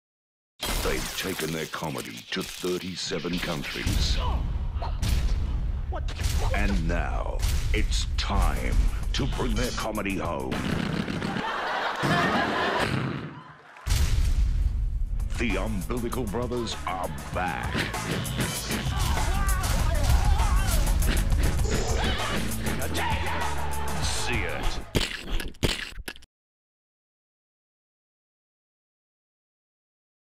The Umbies are coming!
Legendary and live - the Umbilical Brothers are bringing their most beloved show, Speedmouse, to the Theatre this August!